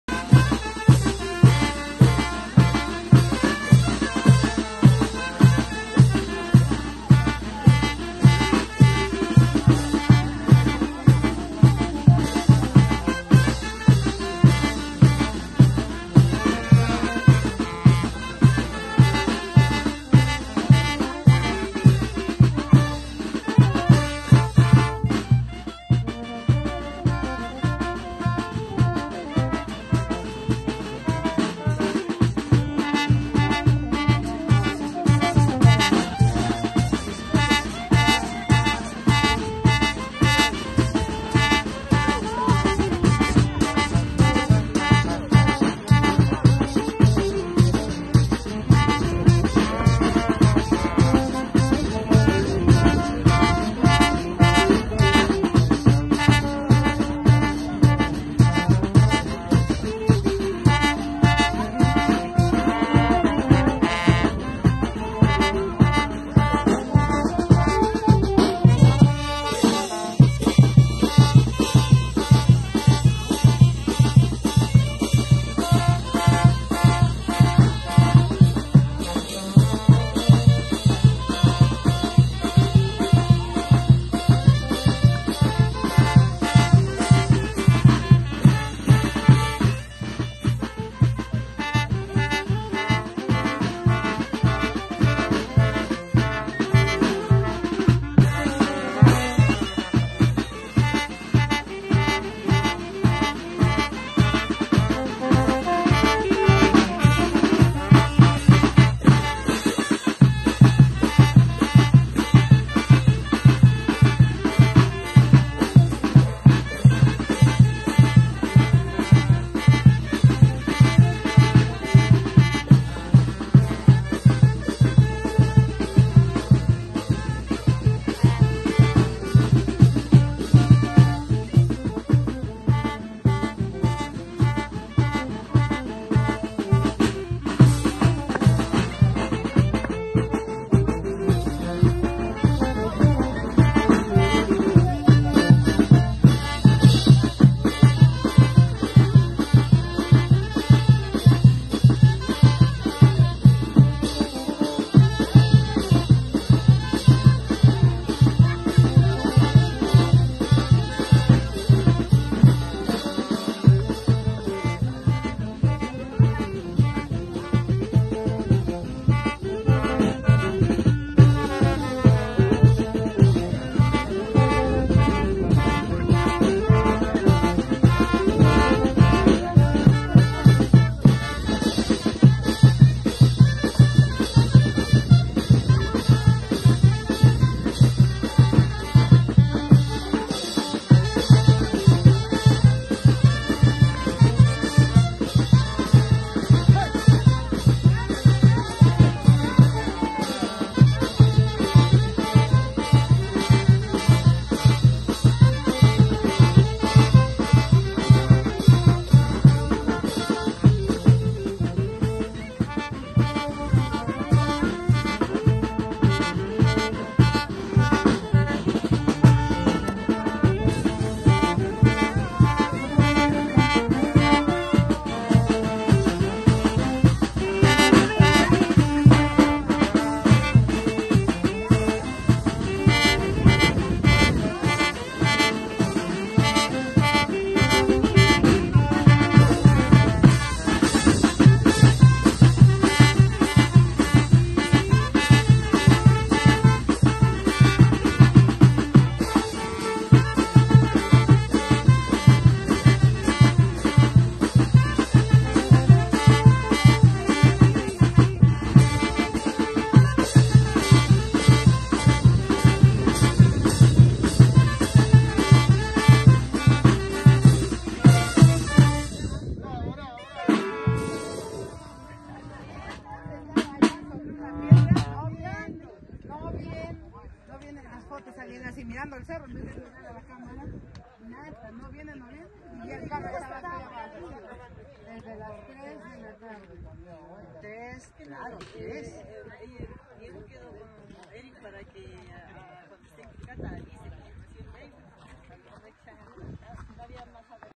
LLEGADA DE COCOLO A PAUZA 2023
Recibiemiento de MAMÁ INCA A SUS HIJOS: COCOLO, ASTRID Y TERESA.
  23 DE MARZO 2023